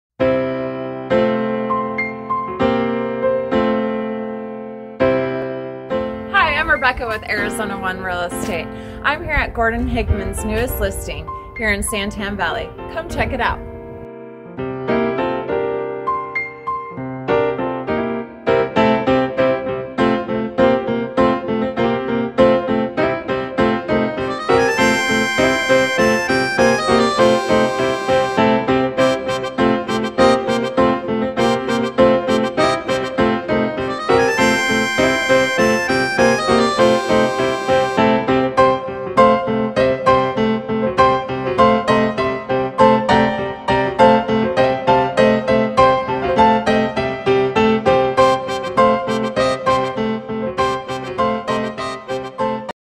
My Johnson Ranch Home - San Tan Valley Homes For Sale
Check out this AWESOME 3 bedroom 2 bathroom home in Johnson Ranch. Looking for the perfect little home or maybe your next investment property? This is the home for you. better hurry though. in this HOT market this will not last!!! Quant open floor concept, large open master suite and HUGE grass backyard with extended patio with plenty of room for the kids to play or your fur babies to roam. Enjoy the community amenities, community pool & nearby golf course. Located at the entry to a cul-de-sac. You will love this adorable home. Don't miss your chance making it yours!

Or

🆓 FREE RESOURCES:
📝 Preferred Arizona Mortgage Partner: Coming Soon